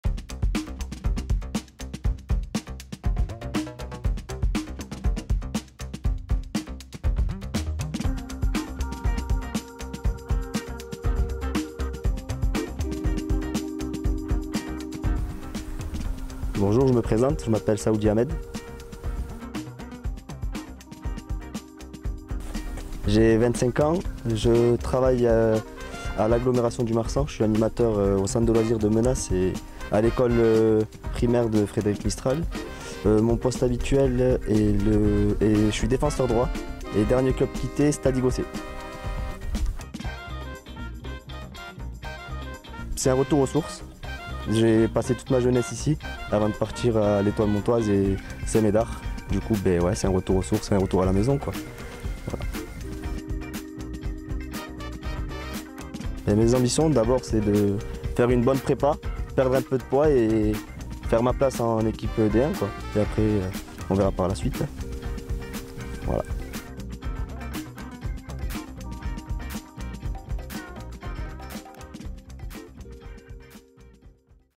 Bienvenue au Sporting Ahmed SAOUDI !
Présentation de Ahmed SAOUDI, joueur sénior, saison 2022/2023 !
Bienvenue au club Ahmed et bonne saison parmi nous !